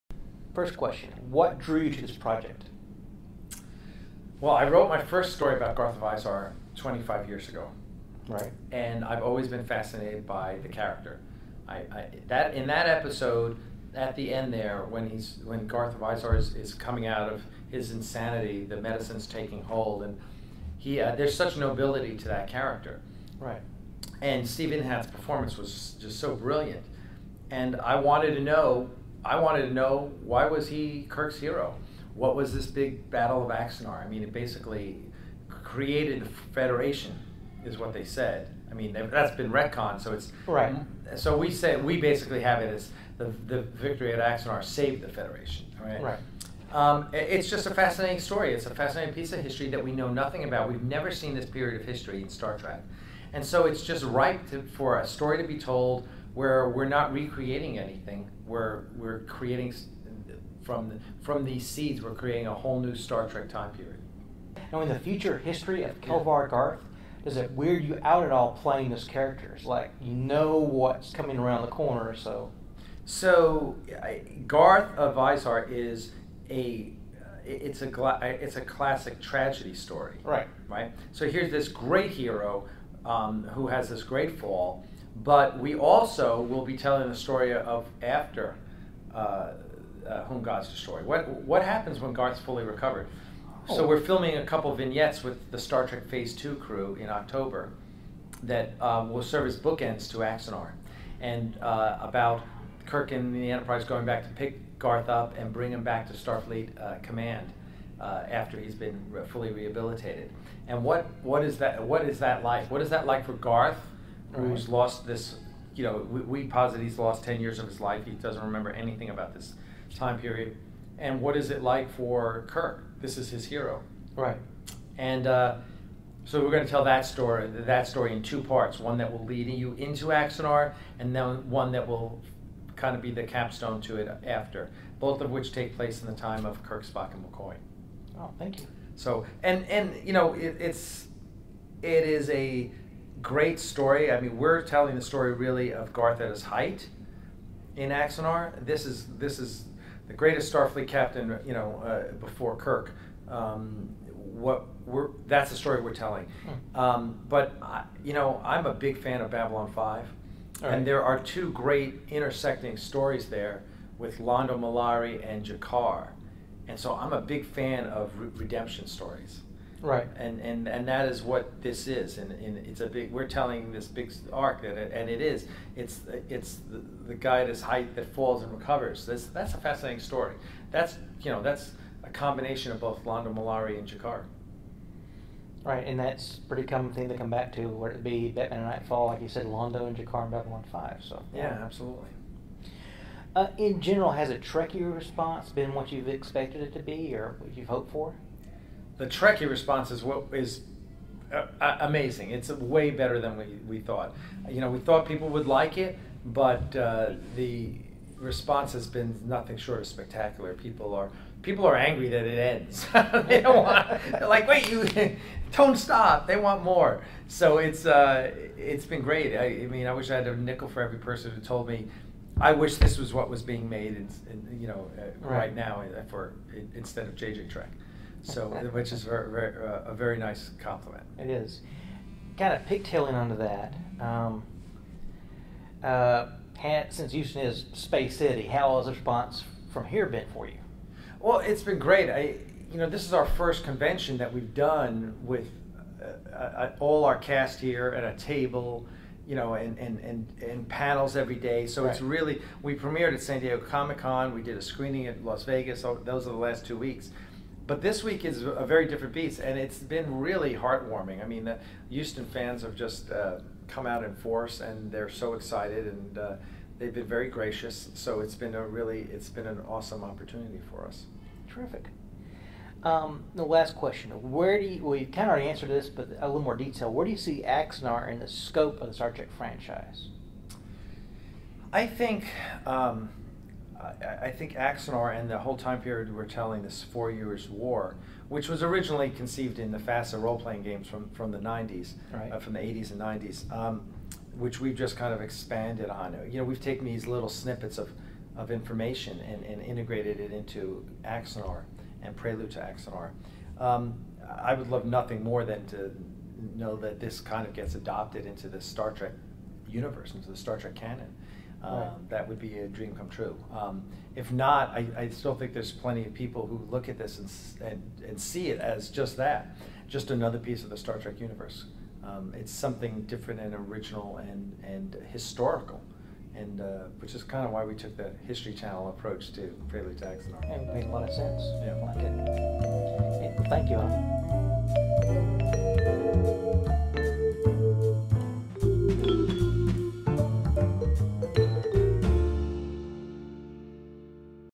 Axanar
An interview with Alec Peters, the producer, and pivotal role in Prelude to Axanar.

Interview at the Houston Con.